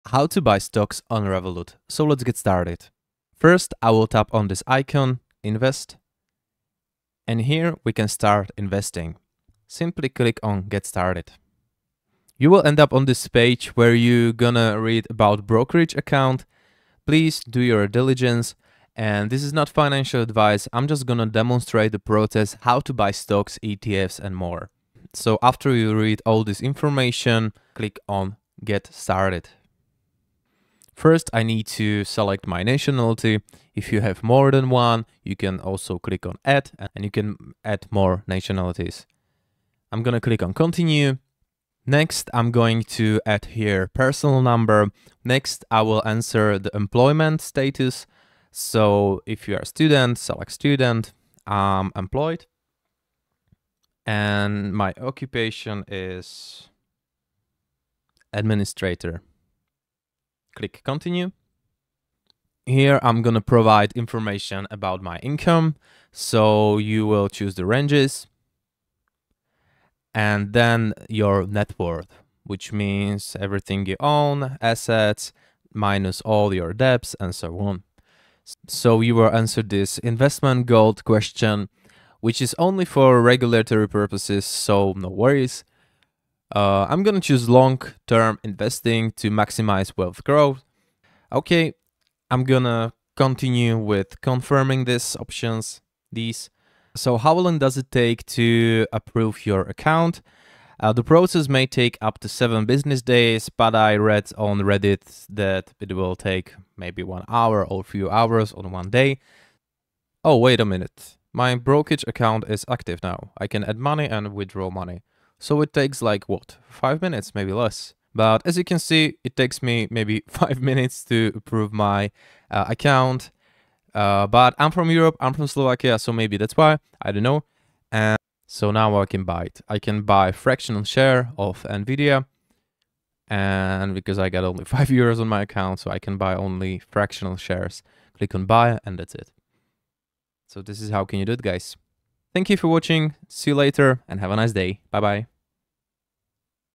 How To Buy Stocks | Revolut
Learn how to buy stocks, ETFs, and more on the Revolut mobile app. In this short tutorial video, I'll show you how to trade stocks and open a brokerage account. I will use only examples, and I am not providing any advice on financial markets.

Useful tools I use every day:

Blue: The best project management tool.